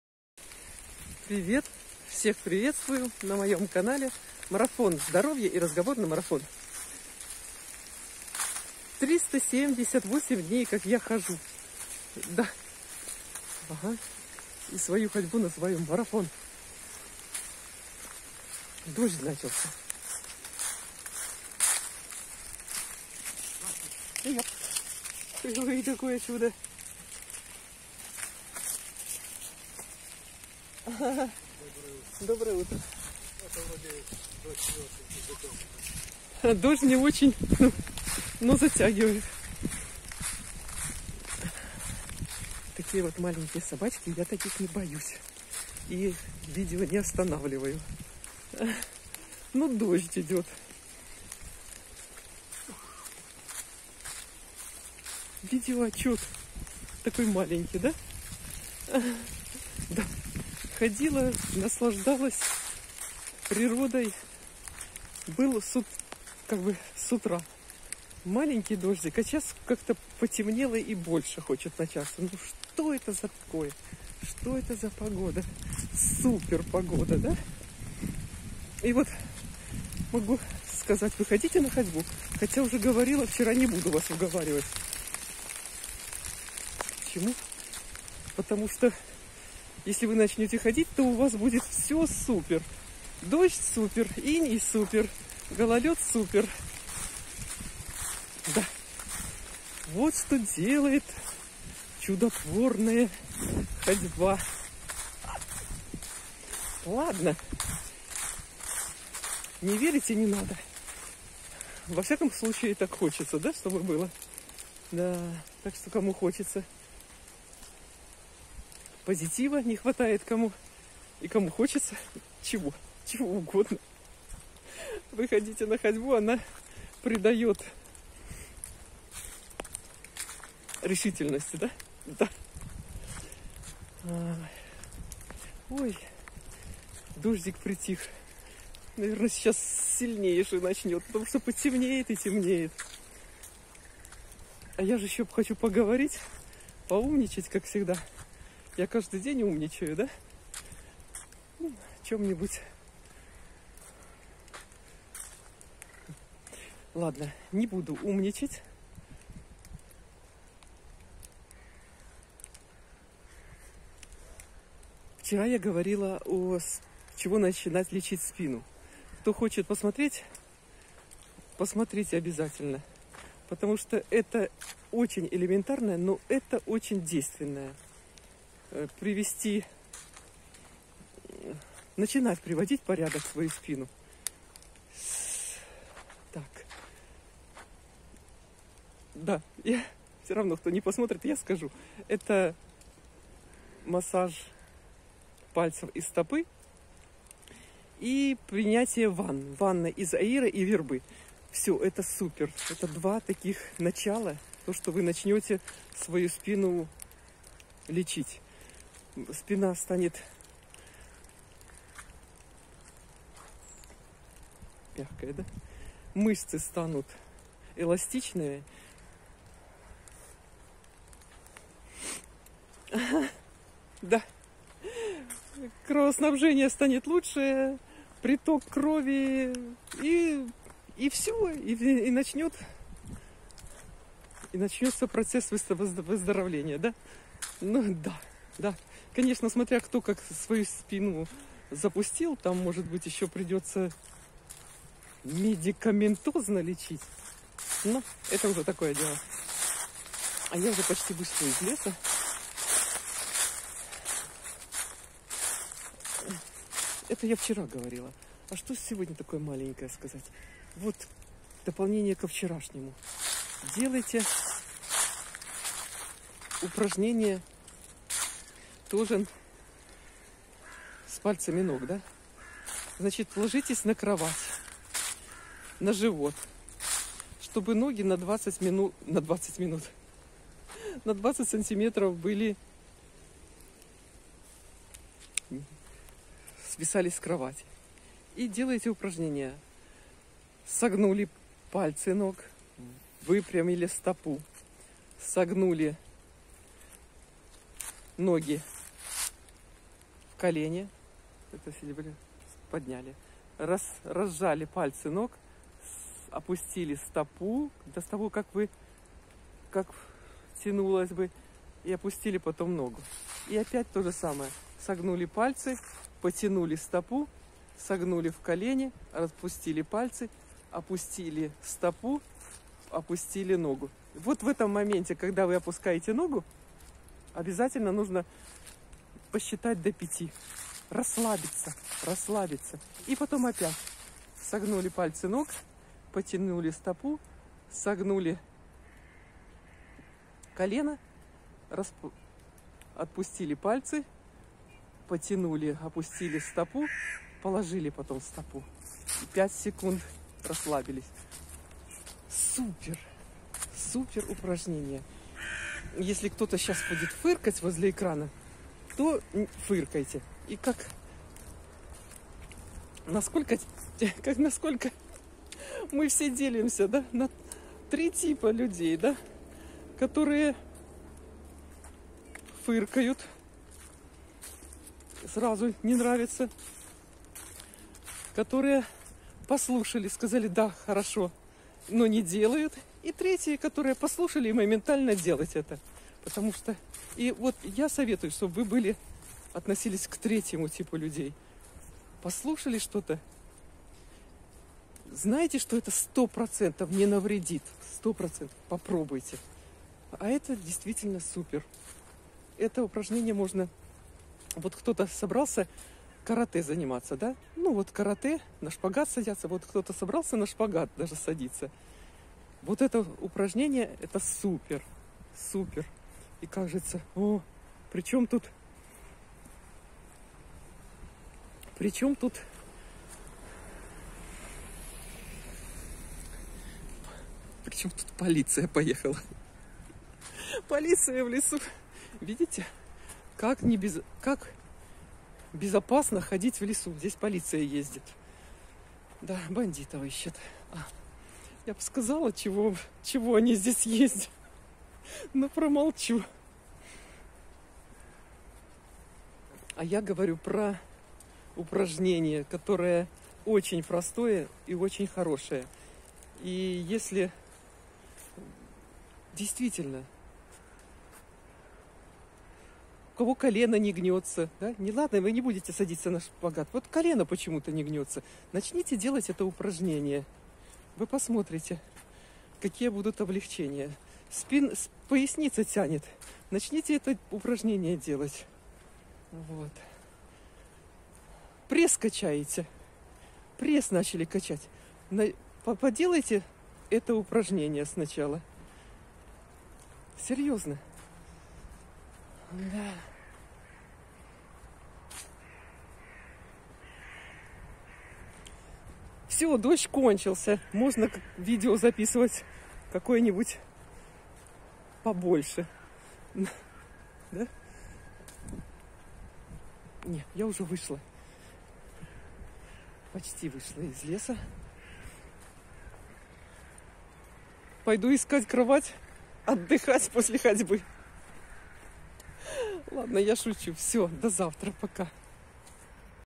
Упражнения для спины. "Марафон здоровья и разговорный марафон" день 378
Всех приветствую на моём канале Марафон здоровья и разговорный марафон. День 378. Дождь не помеха для моей ходьбы. Маленькое дополнение ко вчерашнему разговору. Еще одно упражнение для начала лечения спины.